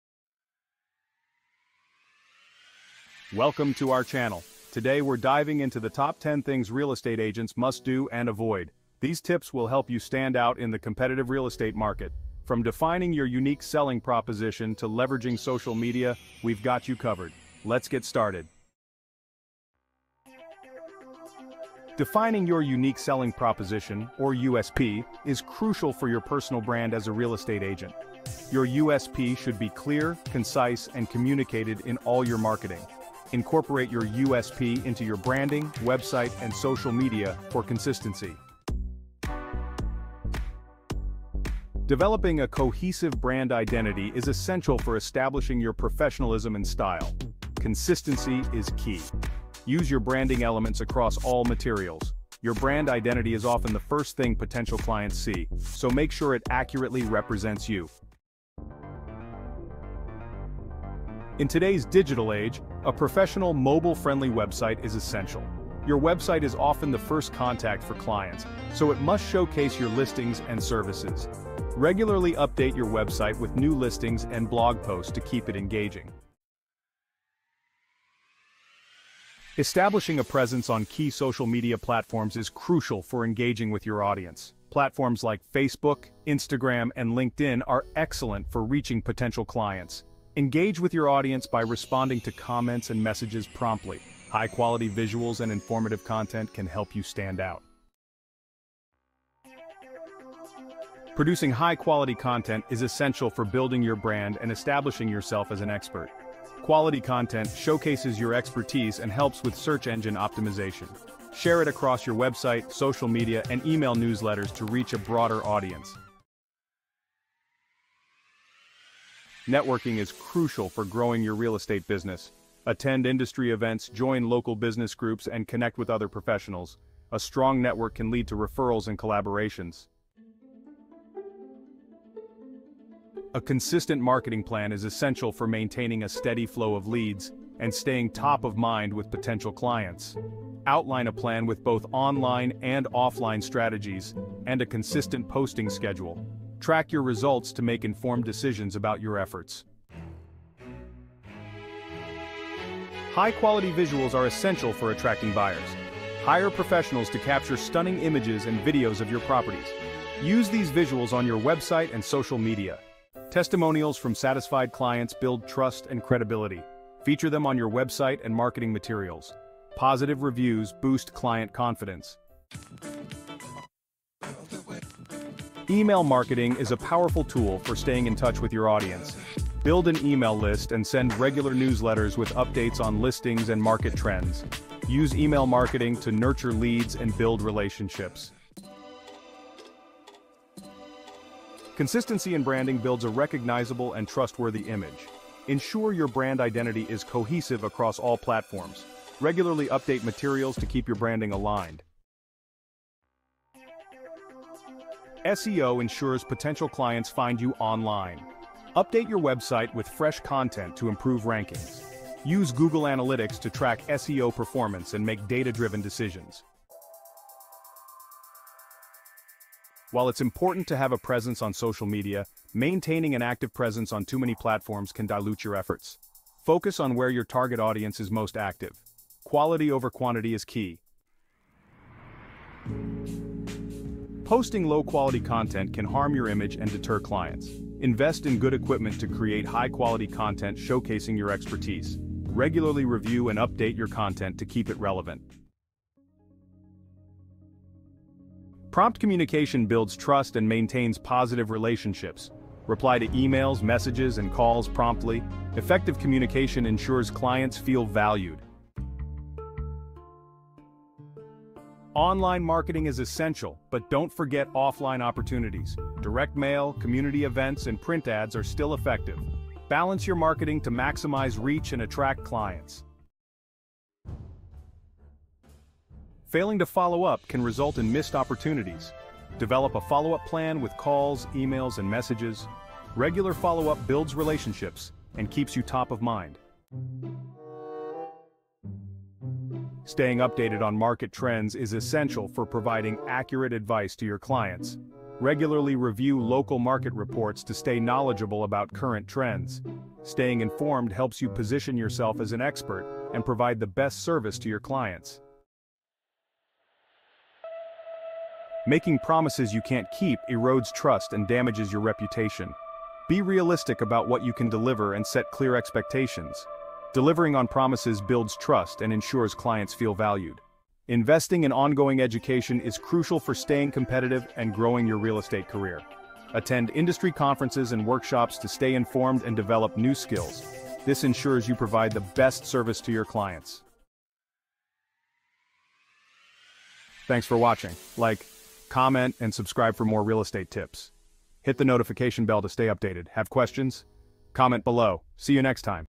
New Realtors the top 10 things you should and should not do
Creating a personal brand and a solid marketing strategy is essential for real estate agents starting out in the industry. It helps establish their identity, build trust with clients, and differentiate themselves from competitors. Here are the top ten things they must do and the top ten things they should not do, with detailed explanations and examples, By following these guidelines and avoiding common pitfalls, new real estate agents can effectively create their own brand and marketing strategy, establishing themselves as trusted professionals in the industry.